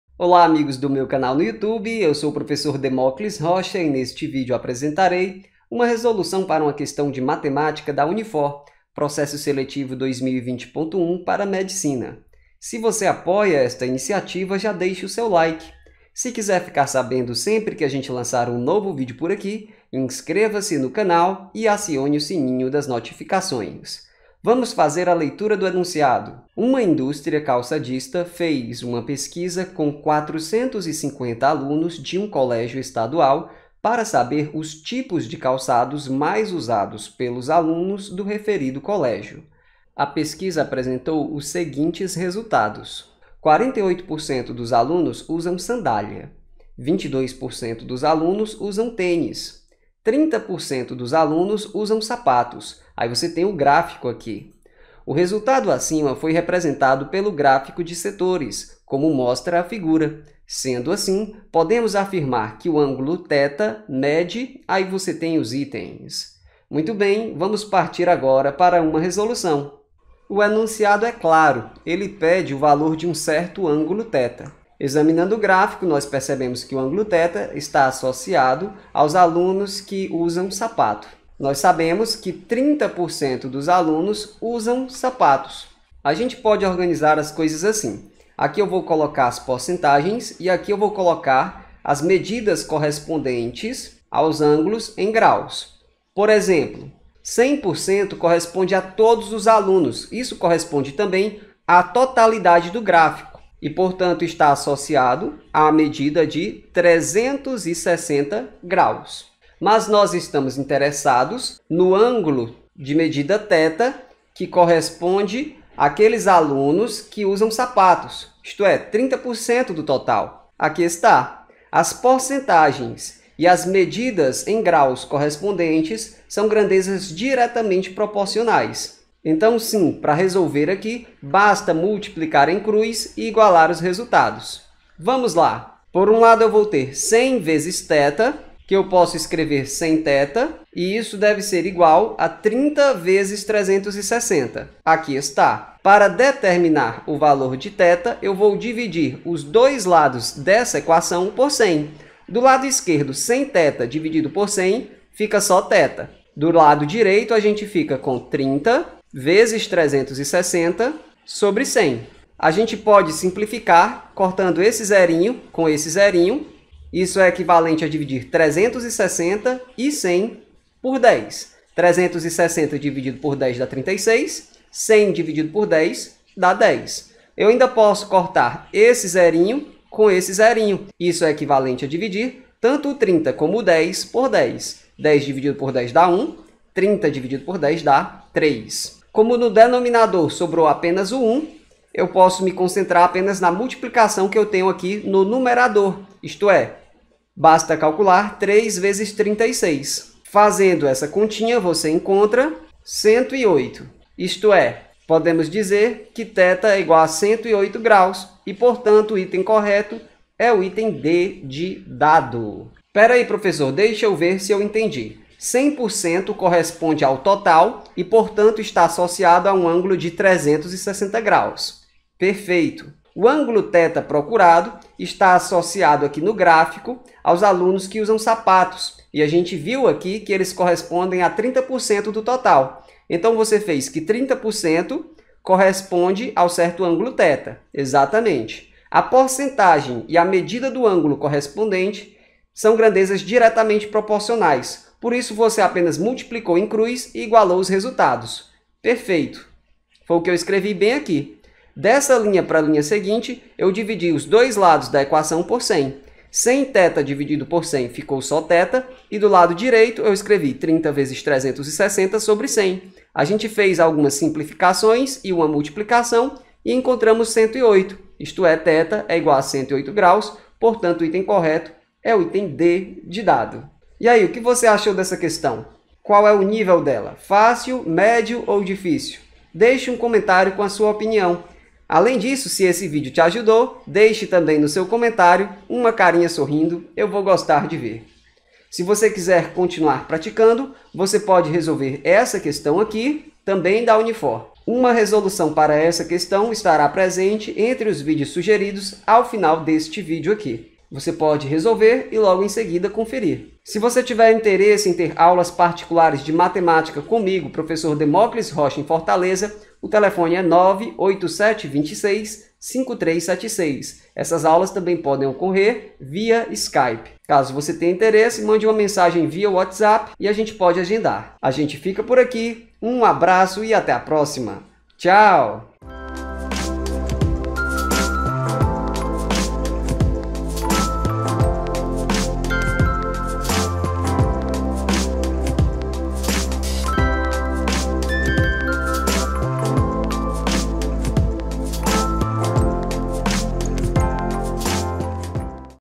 MEDICINA - UNIFOR 2020.1 - QUESTÃO 11 - GRÁFICO DE SETORES
UNIFOR - Processo Seletivo 2020.1 - MEDICINA

MATEMÁTICA E SUAS TECNOLOGIAS

Questão 11

Uma indústria calçadista fez uma pesquisa com
450 alunos de um colégio estadual para saber
os tipos de calçados mais usados pelos alunos
do referido colégio. A pesquisa apresentou os
seguintes dados:
● 48% dos alunos usam sandália.
● 22% dos alunos usam tênis.
● 30% dos alunos usam sapatos

[FIGURA]

O resultado acima foi representado pelo gráfico
de setores, como mostra a figura.Sendo assim,
podemos afirmar que o ângulo θ mede:
(A) 95º
(B) 100º
(D) 108º
(E) 110º

Fortaleza - CE
60840974

Sony Vegas

OBS

MICROFONES